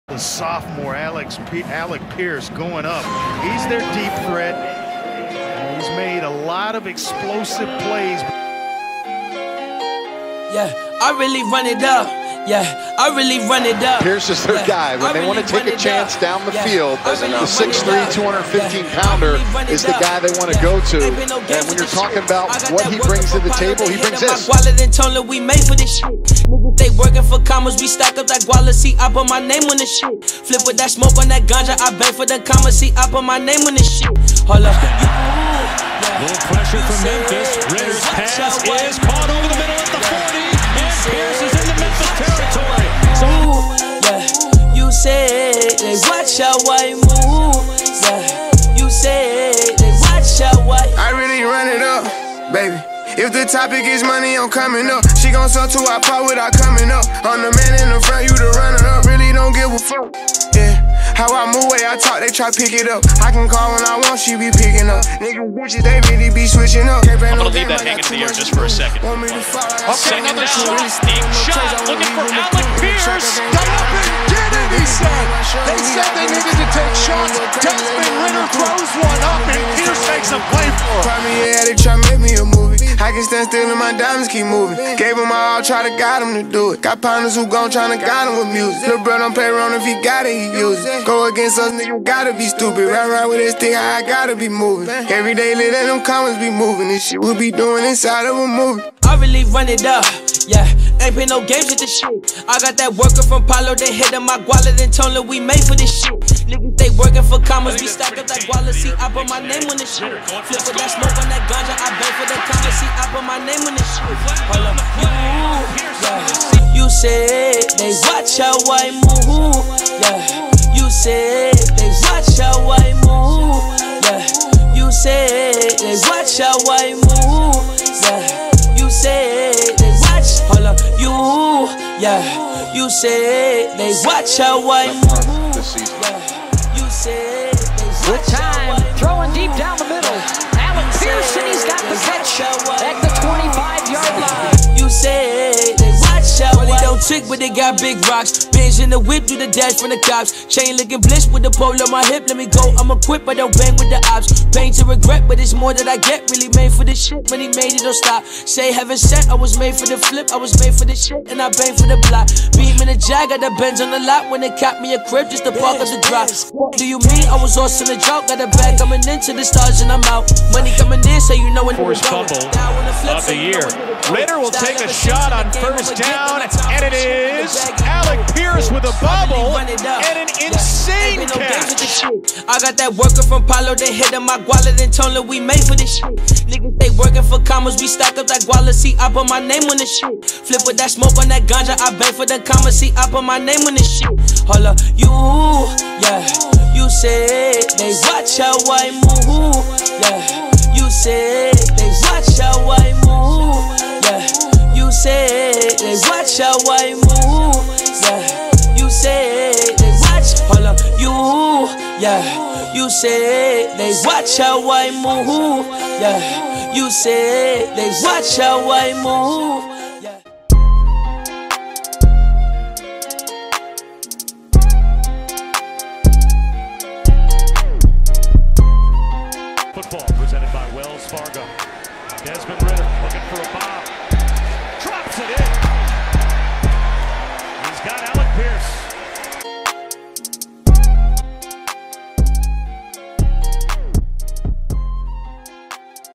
Alec Pierce || UC Bearcats Wide Receiver || 2019 Highlights
Alec Pierce Sophomore season highlights. 652 Receiving Yards, 37 Receptions, 2 Touchdowns.